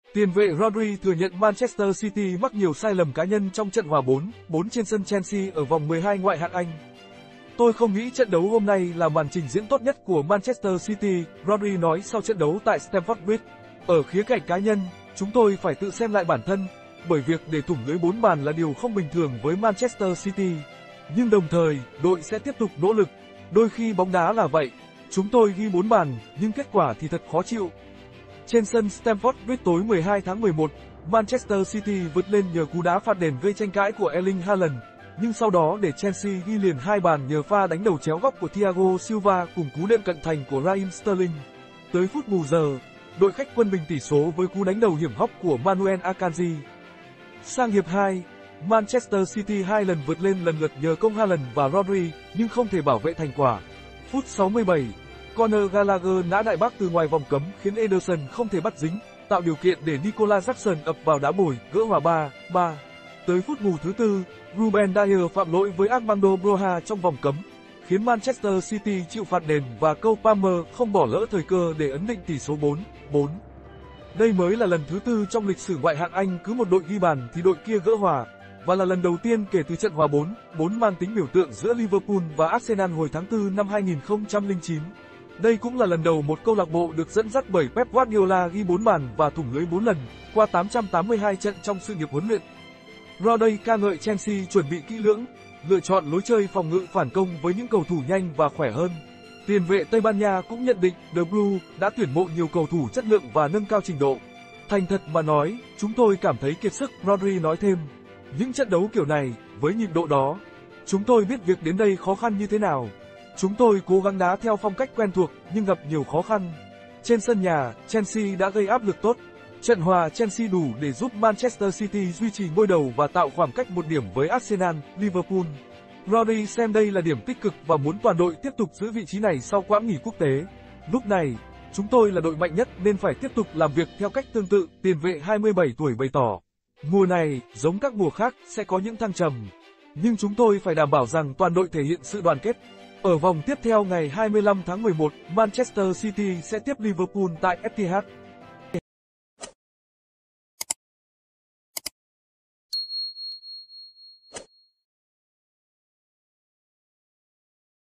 RODRI CHIA SẺ 'MAN CITY THỦNG LƯỚI 4 BÀN TRƯỚC CHELSEA LÀ KHÔNG BÌNH THƯỜNG'
"Tôi không nghĩ trận đấu hôm nay là màn trình diễn tốt nhất của Man City", Rodri nói sau trận đấu tại Stamford Bridge. "Ở khía cạnh cá nhân, chúng tôi phải tự xem lại bản thân, bởi việc để thủng lưới bốn bàn là điều không bình thường với Man City. Nhưng đồng thời, đội sẽ tiếp tục nỗ lực. Đôi khi bóng đá là vậy. Chúng tôi ghi bốn bàn, nhưng kết quả thì thật khó chịu".